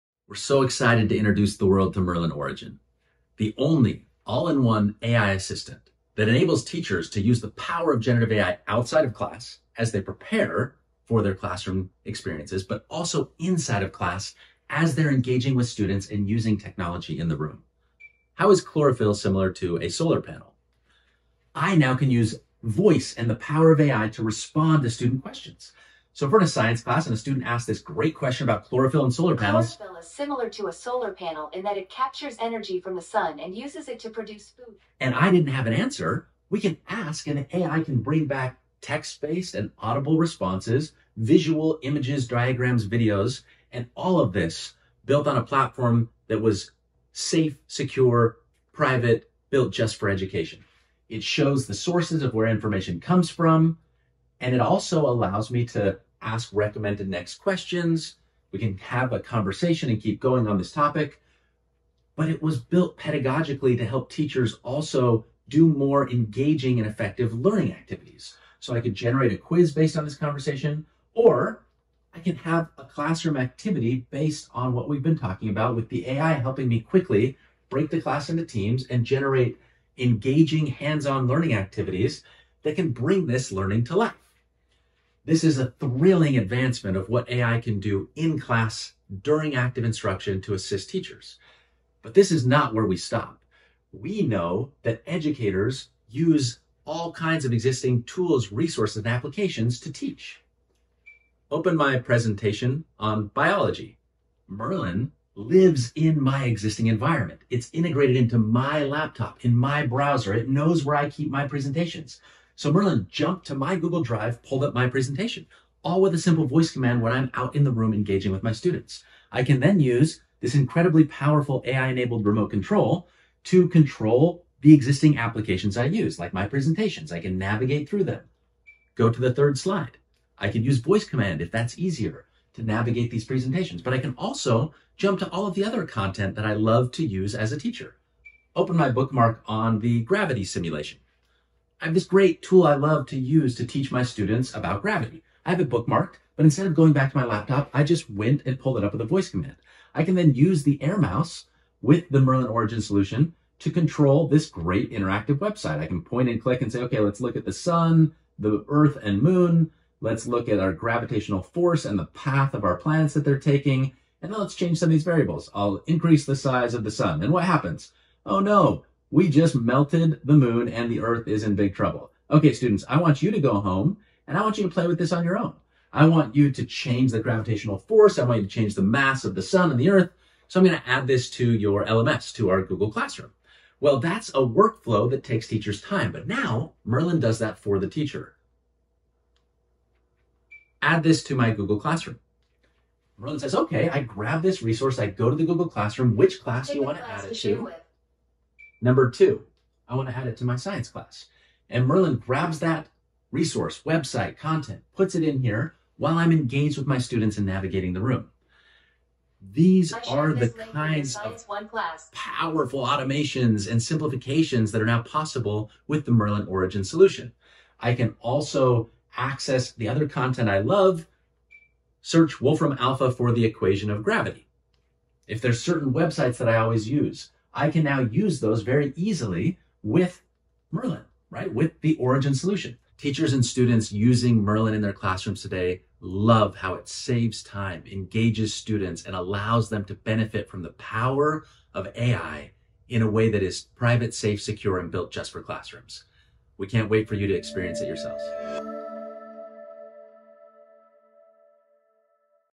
Merlyn Origin – The all-in-one AI assistant for educators
Merlyn is the all-in-one AI assistant for education. During teacher prep outside of class, Merlyn supercharges productivity. During active instruction in class, Merlyn’s chat experience and voice control of technology frees teachers to focus on what matters most: their students.